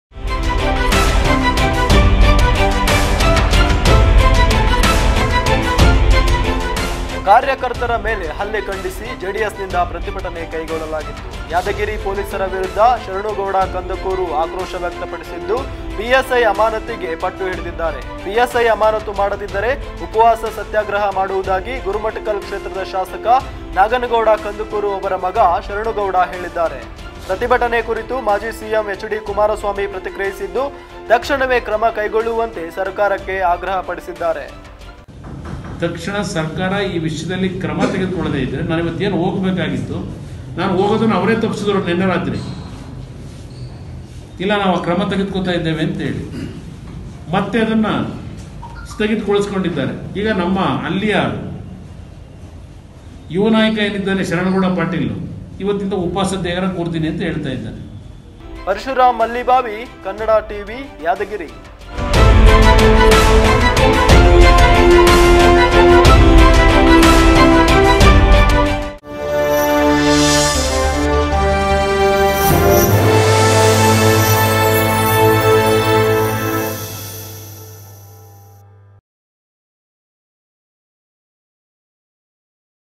ಕಾರ್ಯಕರ್ತರ ಮೇಲೆ ಹಲ್ಲೆ ಖಂಡಿಸಿ ಜೆಡಿಎಸ್ನಿಂದ ಪ್ರತಿಭಟನೆ
ಯಾದಗಿರಿ ಪೋಲೀಸರ ವಿರುದ್ಧ ಶರಣಗೌಡ ಕಂದಕೂರು ಆಕ್ರೋಶ
ಪ್ರತಿಭಟನೆ ಕುರಿತು ಪ್ರತಿಕ್ರಿಯಿಸಿದ ಮಾಜಿ ಸಿಎಂ ಕುಮಾರಸ್ವಾಮಿ